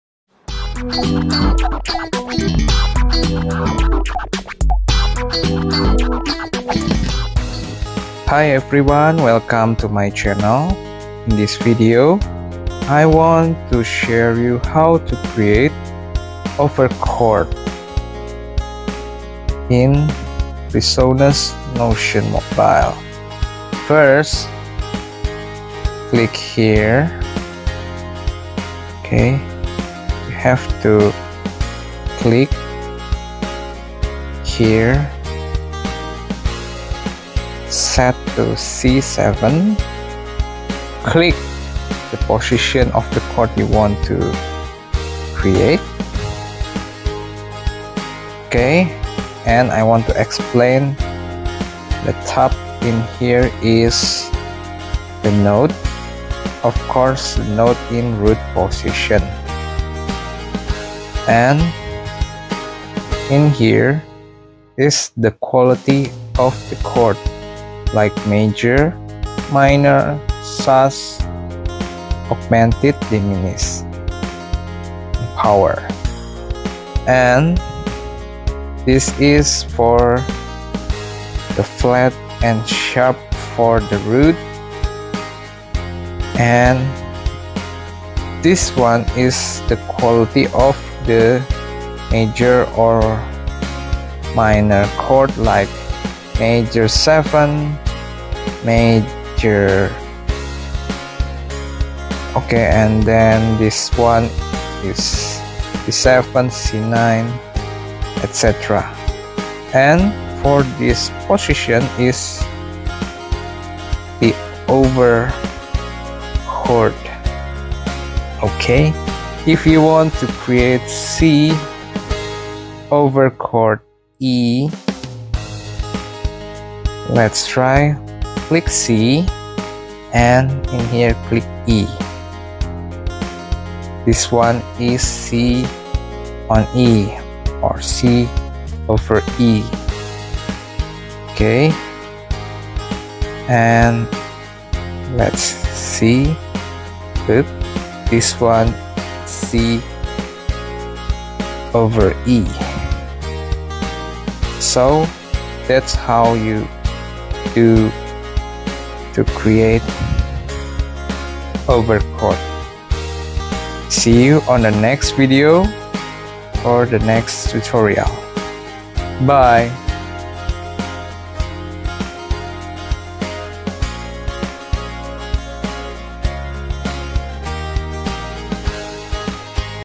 How to Create Over Chord in Presonus Notion Mobile
In this video, I want to share How to Create Over Chord in Presonus Notion Mobile.